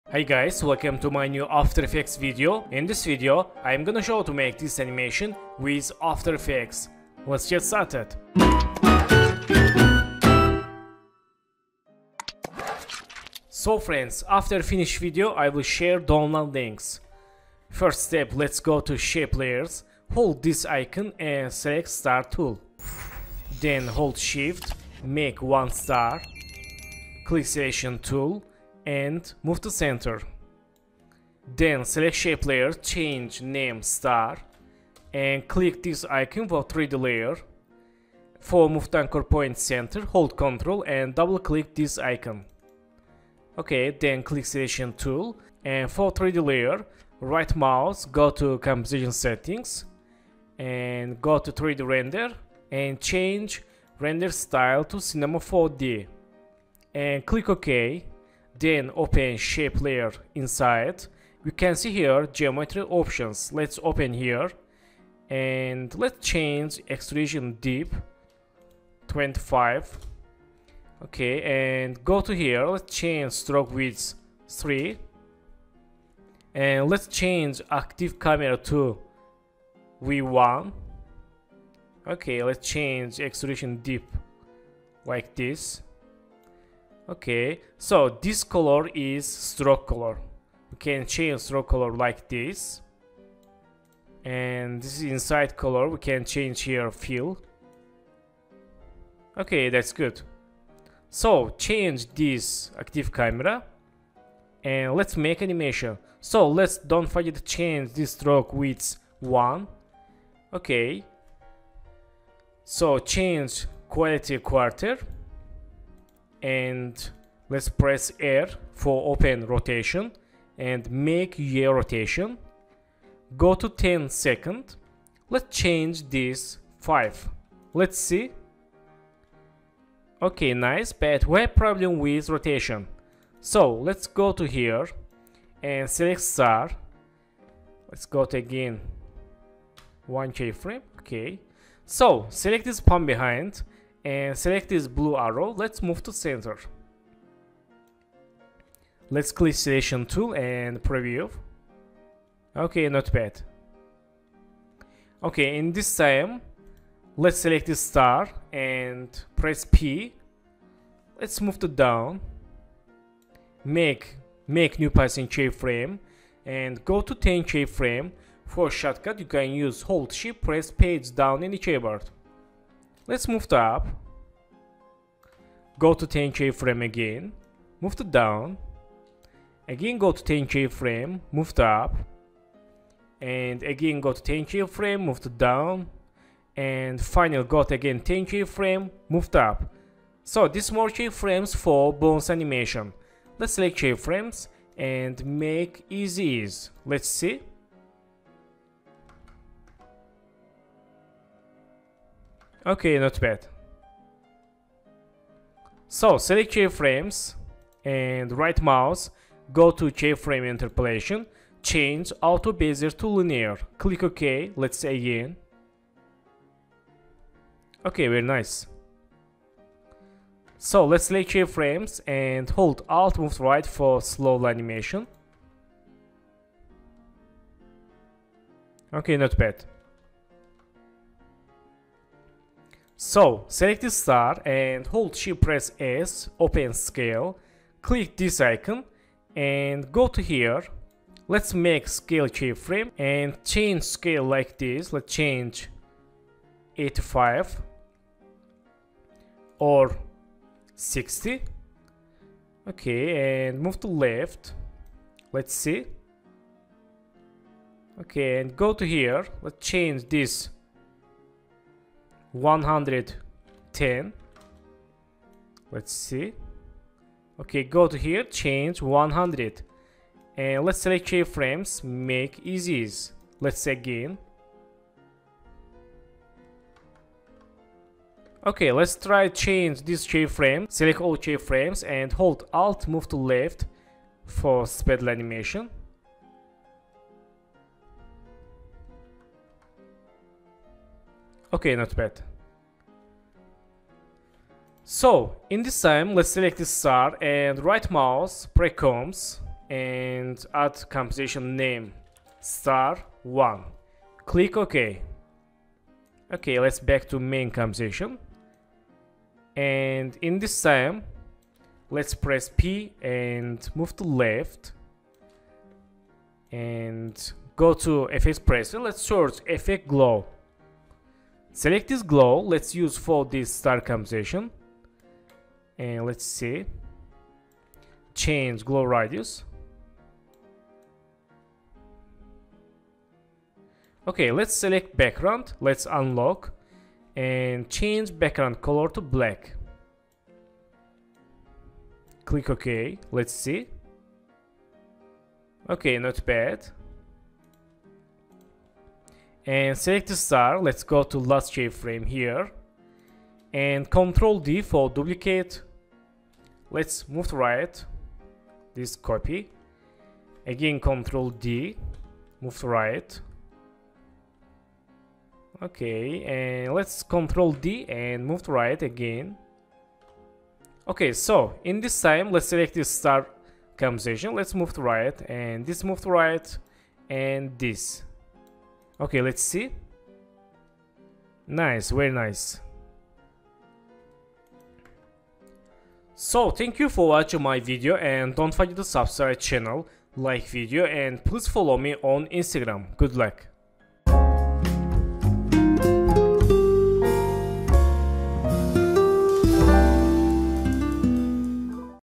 3D Review Stars Animation in After Effects Tutorials | NO PLUGIN
Hi guys, welcome to my new After Effects tutorial. In this tutorial i will show how to make 3D review stars animation with After Effects. Let's get started!